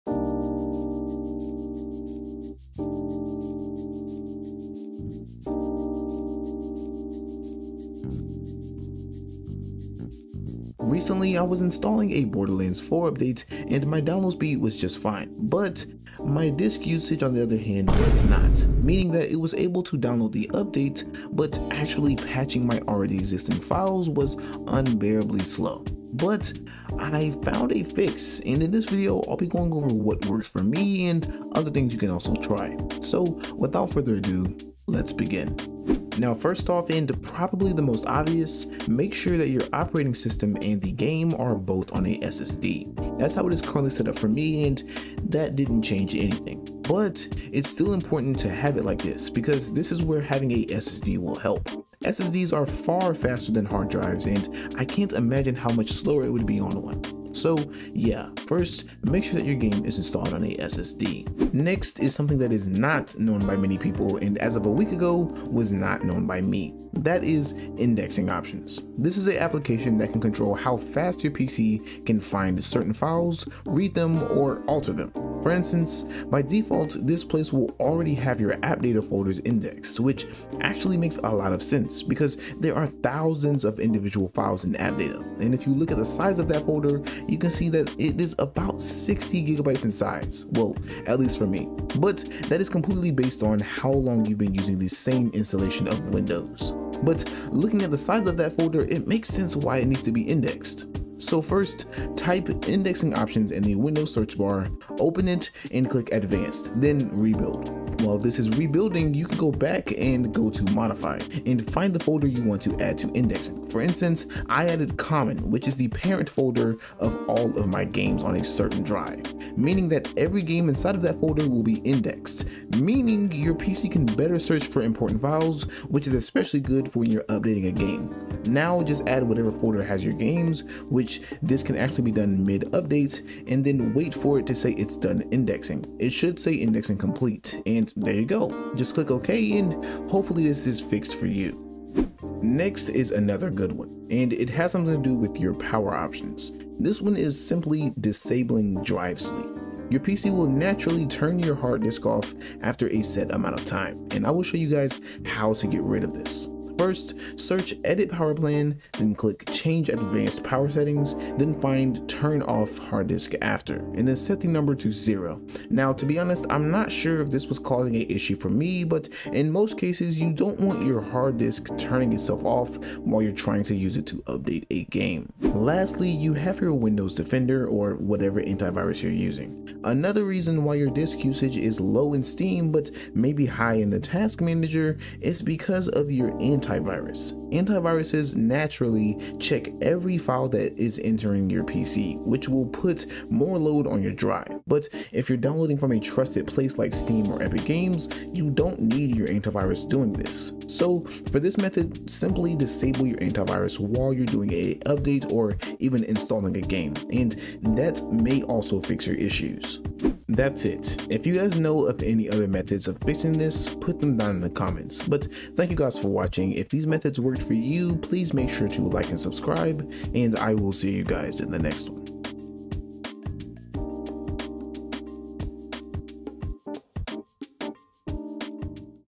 Why Steam Downloads Are So Slow (and How to Fix It)
In this video, I’ll show you how to fix low disk usage in Steam by optimizing your PC’s drive settings and Windows indexing. You’ll learn how to make Steam fully utilize your disk speed and improve overall performance on both SSDs and HDDs.